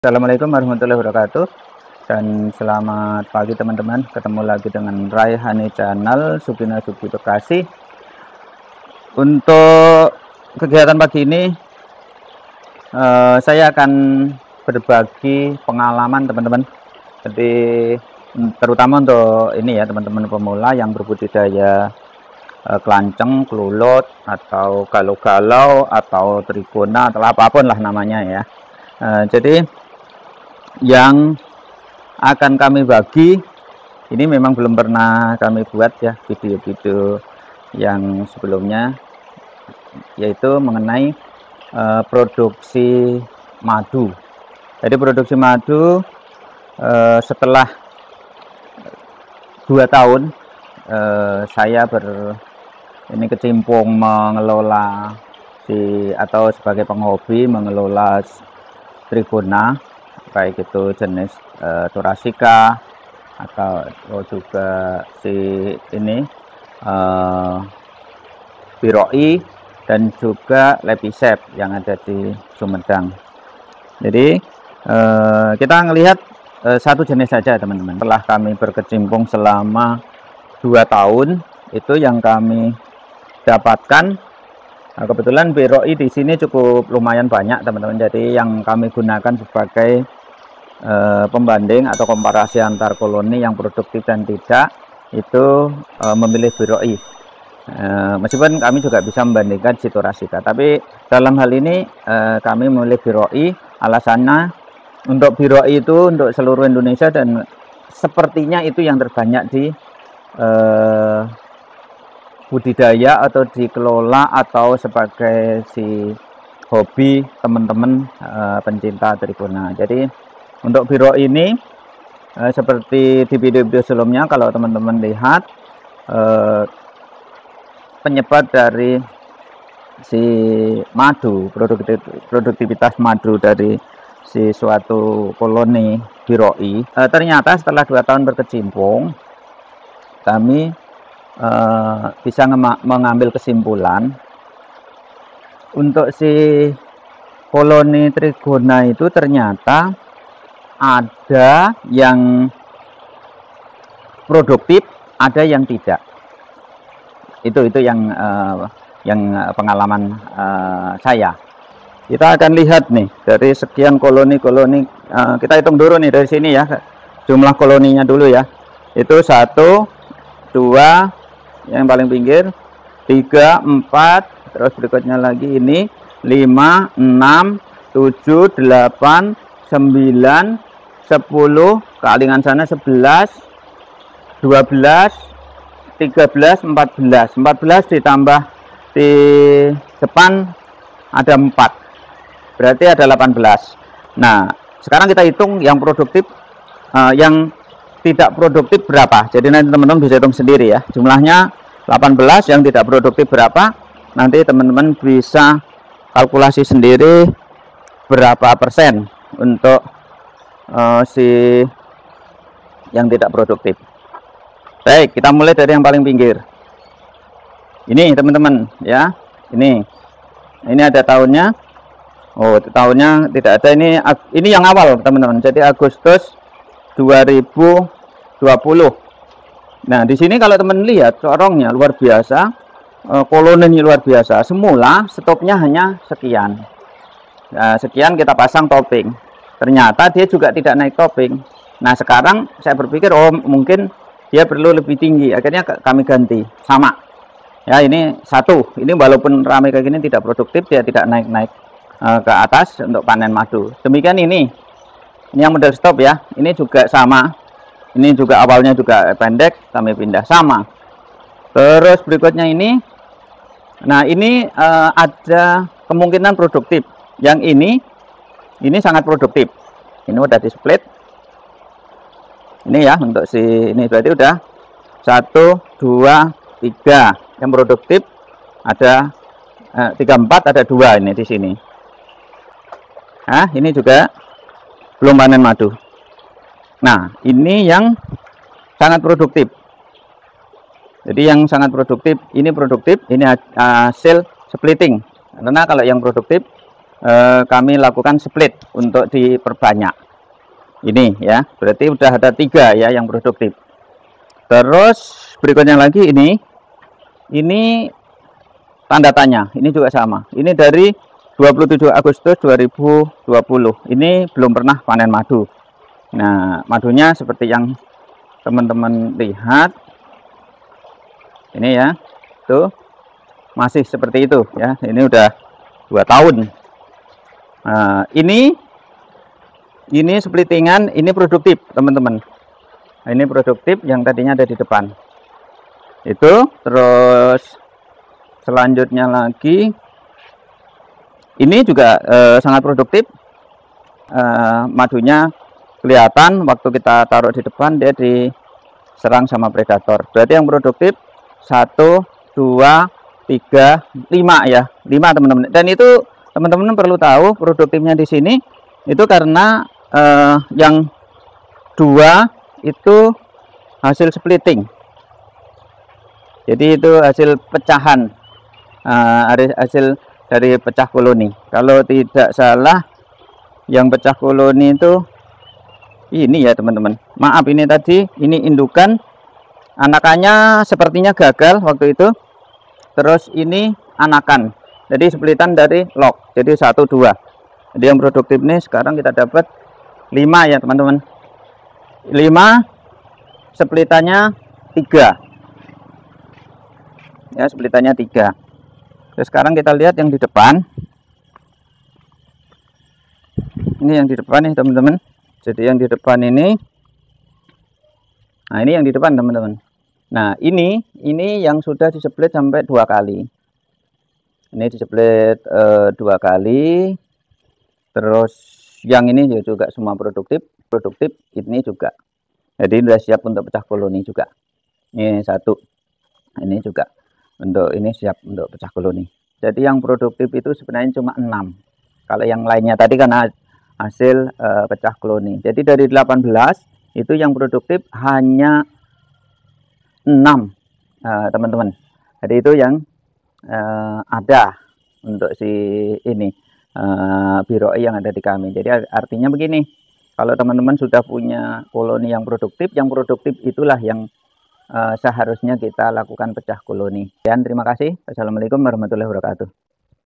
MENGHITUNG KOLONI BIROI TIDAK PRODUKTIF
Video ini berisi Tutorial Budidaya Lebah Trigona Biroi, Tutorial Tetragonula Biroi, Cara Budidaya Trigona Biroi dengan Referensi Pengalaman Selama 2 tahun lebih memelihara lebah Trigona Biroi yang berasal dari Bone maupun Sopeng Sulawesi, Video ini terdiri beberapa bagian dari persiapan, pemidahan (untuk pemindahan disini pecah koloni dari log yang sudah dipersiapkan ini koloni yang pernah ratunya diambil), koloni, perawatan dan panen madu.

Terimakasih
Ray Honey Channel

LINK TERKAIT:

tag;